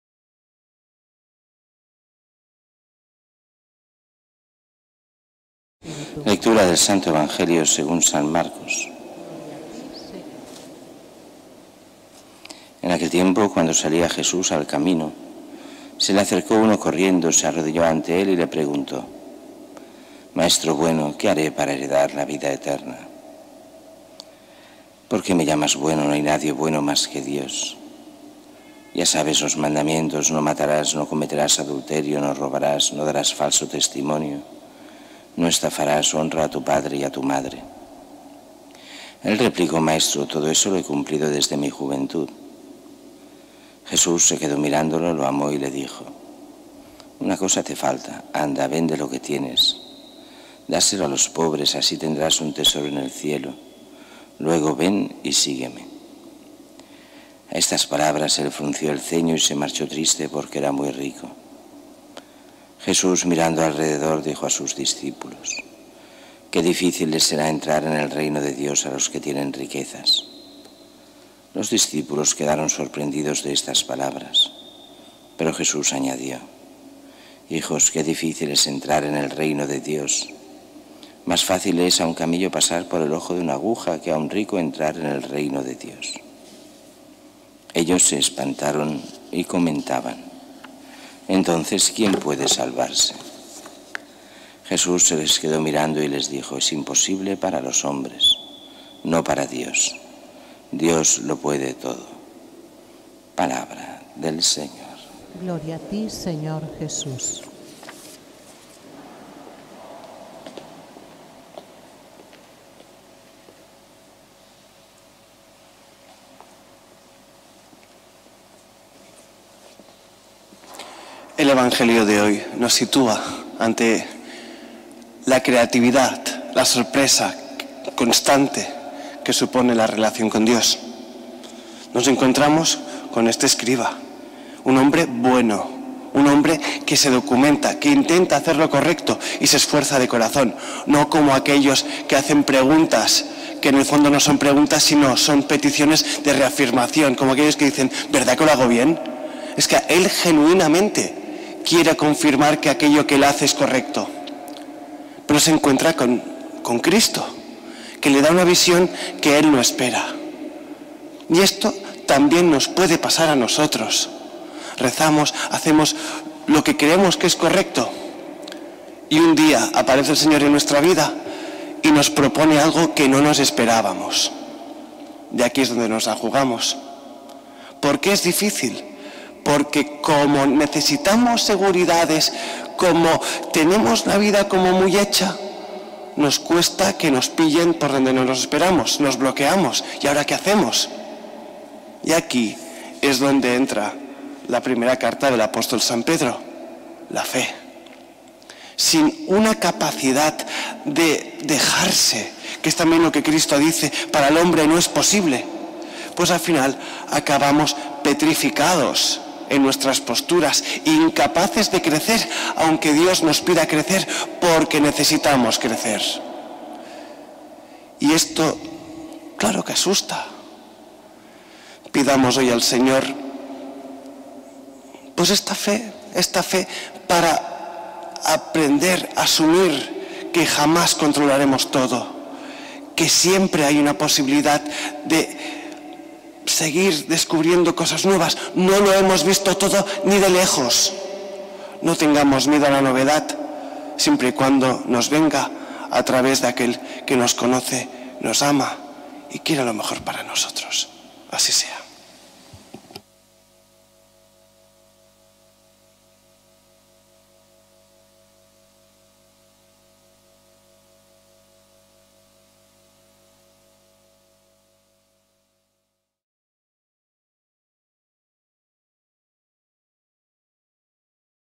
Evangelio y homilía de hoy lunes 28 de febrero de 2022
Nazaret TV te ofrece el evangelio y la homilía de la santa misa de hoy,  lunes 28 de febrero de 2022
Desde la Parroquia de santa Teresita del Niño Jesús, en Barcelona.
Predica: Rvdo. Vicenç Martí, pbro. Lector: Rvdo. Enric Ribas, pbro.
Día litúrgico: Lunes de la VIII semana del tiempo ordinario.
El evangelio de hoy corresponde a san Marcos 10, 17-27